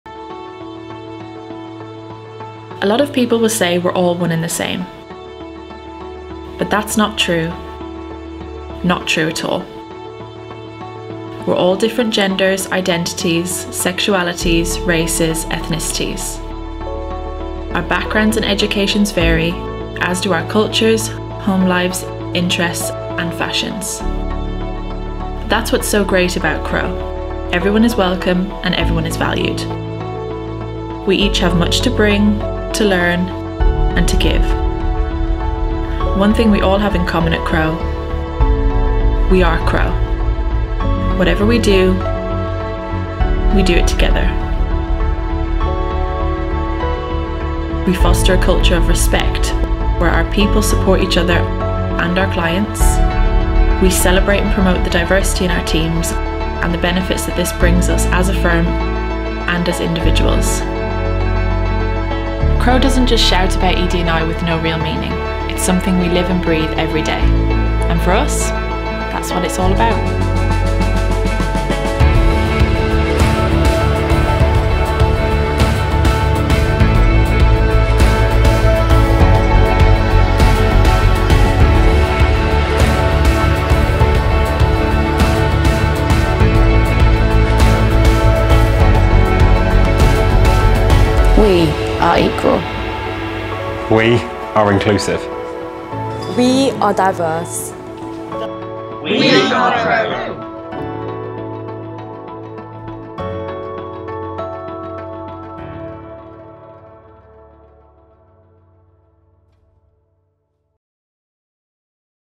National Inclusion Week 2023
We are proud to come together to celebrate the diversity of our teams and to foster a culture where our people feel respected and valued for who they are.

Our differences are what makes each of us unique. We love to listen and develop our knowledge and awareness of what makes us all different.

At Crowe, we encourage and support our people to bring their whole selves to work.

We are equal ⭐. We are inclusive 💕. We are diverse 🎨. We are Crowe 💛.